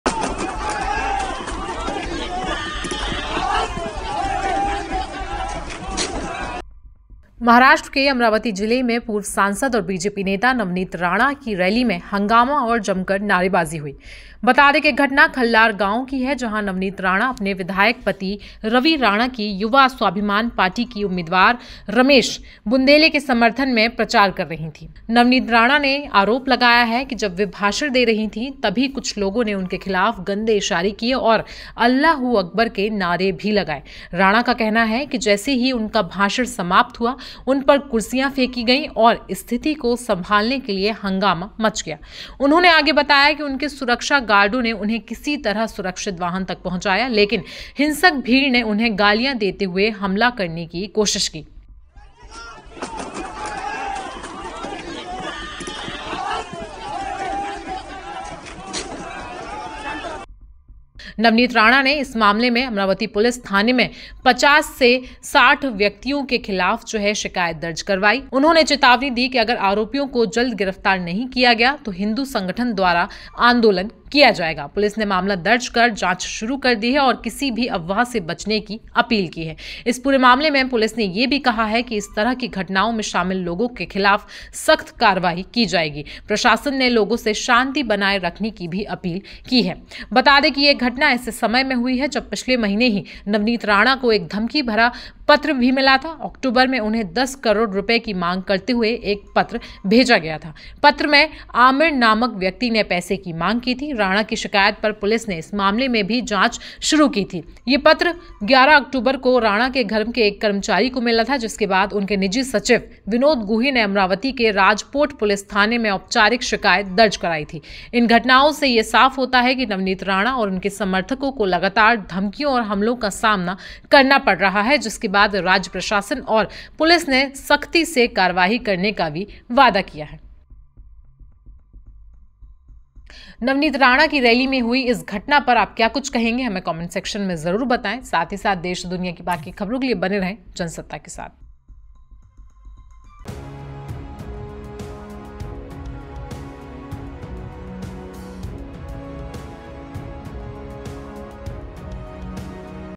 Navneet Rana Attack: रैली में नवनीत राणा पर हुई हमला करने की कोशिश, पुलिस ने दर्ज की शिकायत
Navneet Rana Rally: महाराष्ट्र (maharashtra) के अमरावती जिले में पूर्व सांसद और बीजेपी नेता नवनीत राणा की रैली में हंगामा और जमकर नारेबाजी हुई। घटना खल्लार गांव की है जहां नवनीत राणा अपने विधायक पति रवि राणा की युवा स्वाभिमान पार्टी के उम्मीदवार रमेश बुंदिले के समर्थन में प्रचार कर रही थीं। नवनीत राणा (navneet rana) ने आरोप लगाया कि जब वे सभा में भाषण दे रही थीं, तभी कुछ लोगों ने उनके खिलाफ गंदे इशारे किए और 'अल्लाह हू अकबर' के नारे लगाए। राणा का कहना है कि जैसे ही उनका भाषण समाप्त हुआ, उन पर कुर्सियां फेंकी गईं और स्थिति को संभालने के लिए हंगामा मच गया। उन्होंने बताया कि उनके सुरक्षा गार्डों ने उन्हें किसी तरह सुरक्षित वाहन तक पहुंचाया, लेकिन हिंसक भीड़ ने उन्हें गालियां देते हुए हमला करने की कोशिश की।